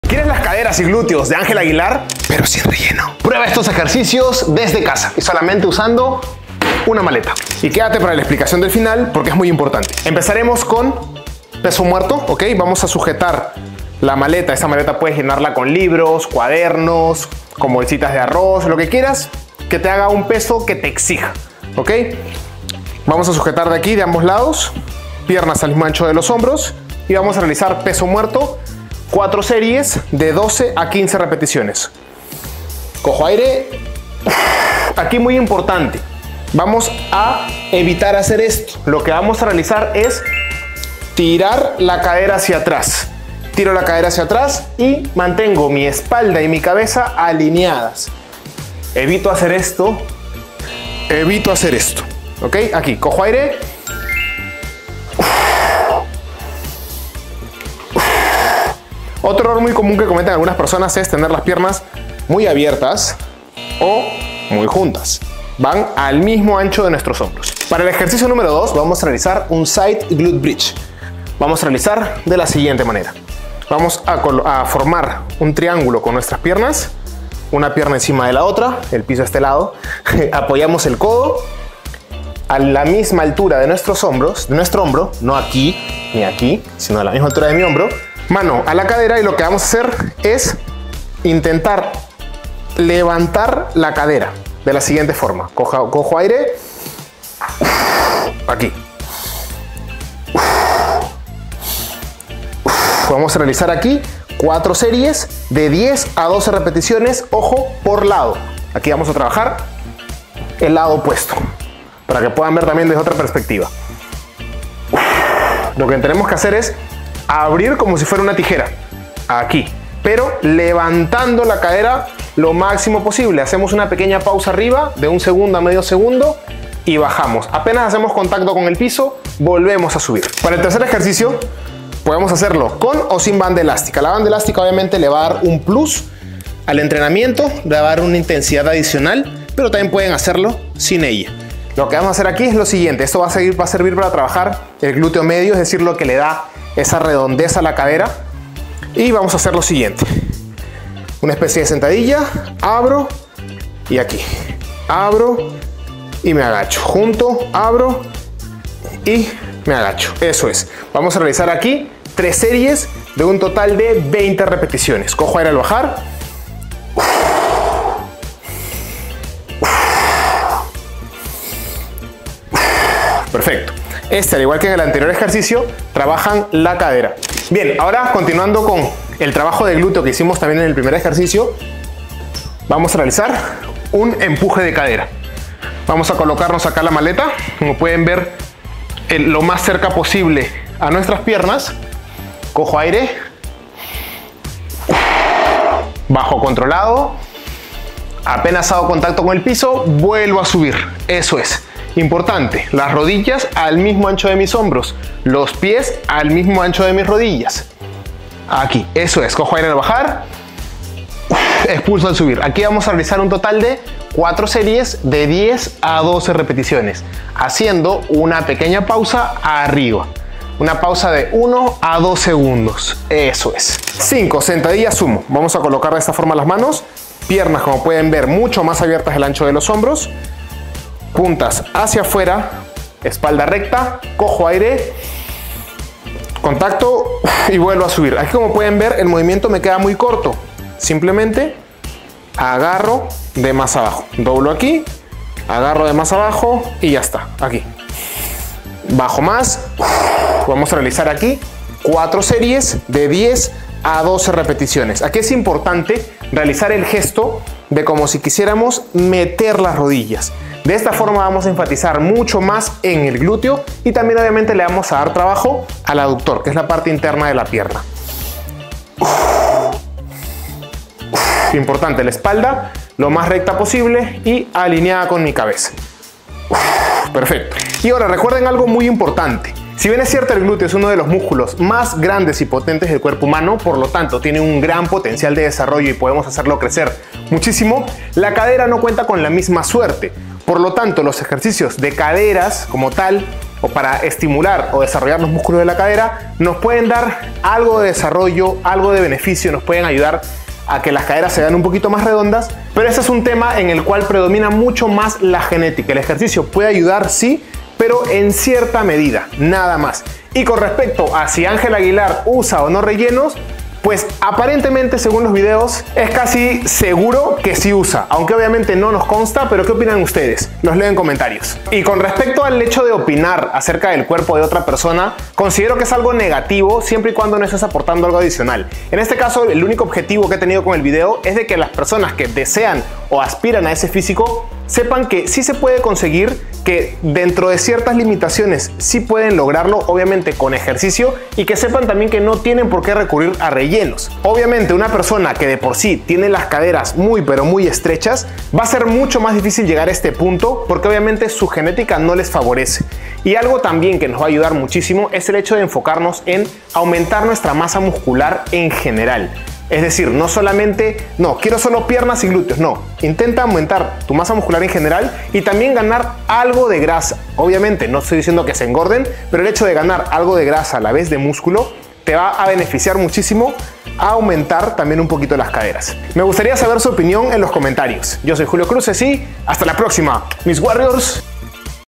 Rutina de ejercicios de GLÚTEOS y CADERAS de ÁNGELA AGUILAR | Cadera y glúteos en casa🏡
En este nuevo video les muestro la RUTINA de Ángela Aguilar para mejorar sus GLÚTEOS y CADERAS, estos ejercicios se pueden hacer desde CASA!! Mira el video y hacé un cambio físico hoy